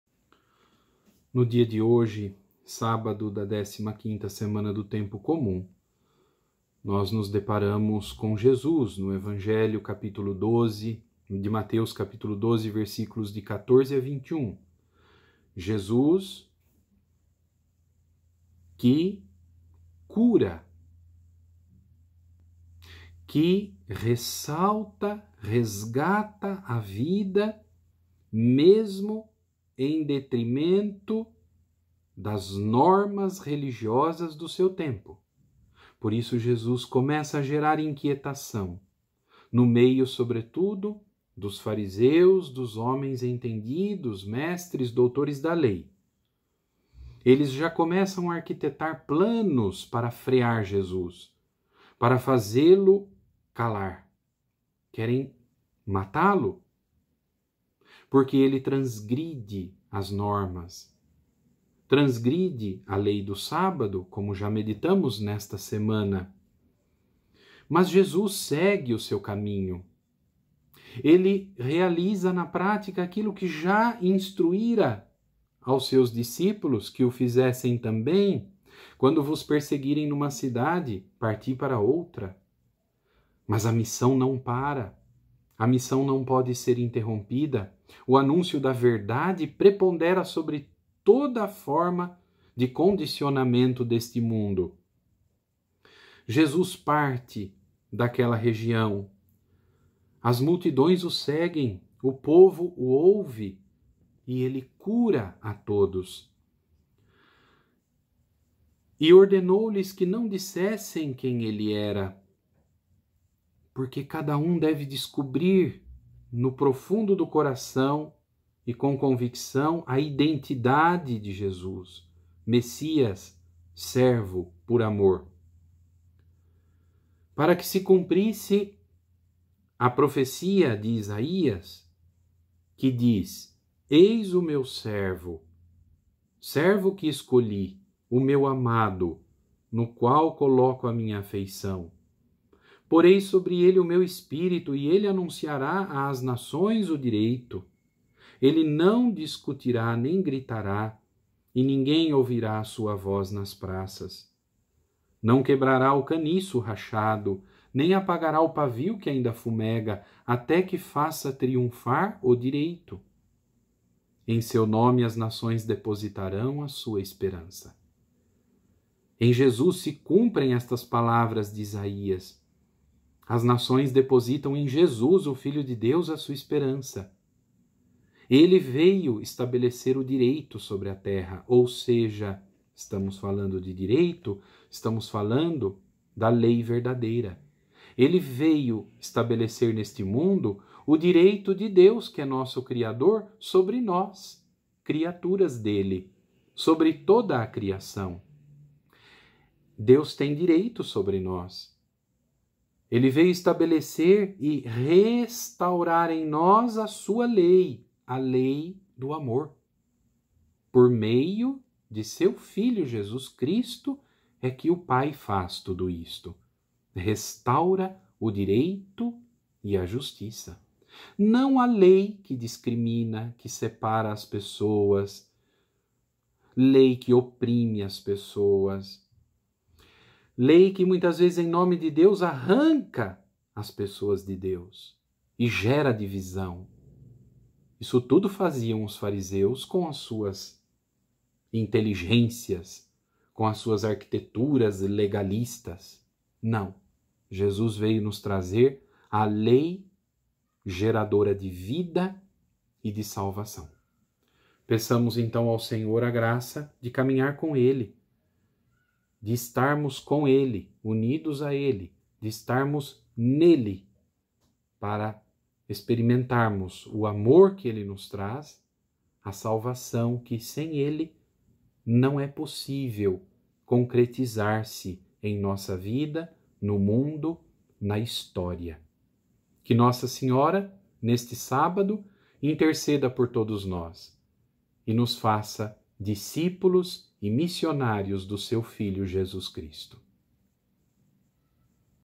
Meditação diária - Monsenhor Rafael Capelato - 18/07/2020
Meditação diária feita pelo Monsenhor Rafael Capelato, pároco da Catedral Metropolitana de Campinas.